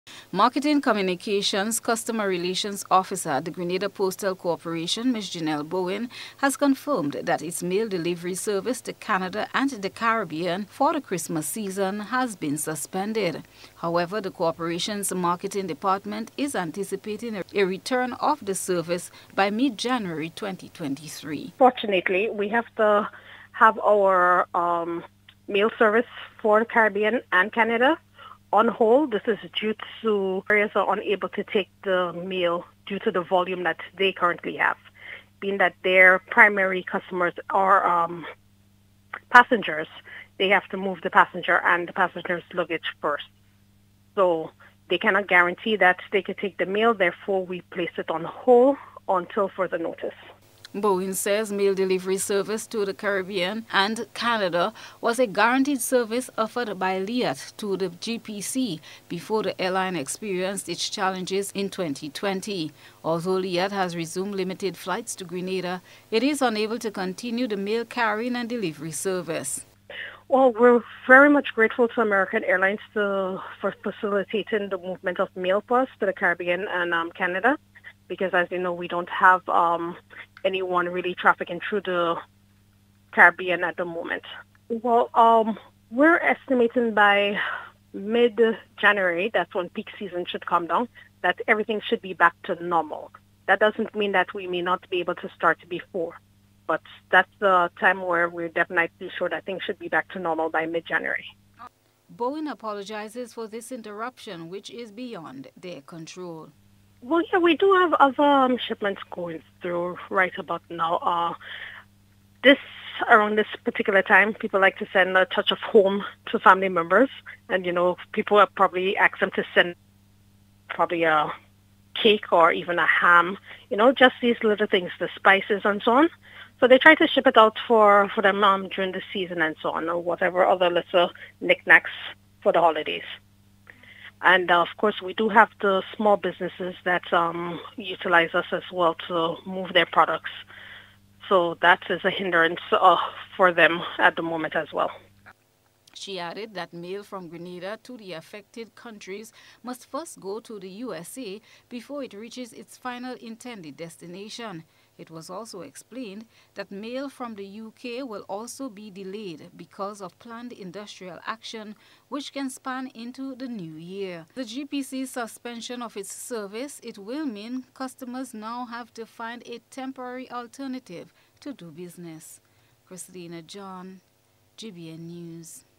MAIL SERVICE DELIVERY DISRUPTED
Mail delivery from Grenada faces interruption but it is anticipated that this will be short-lived.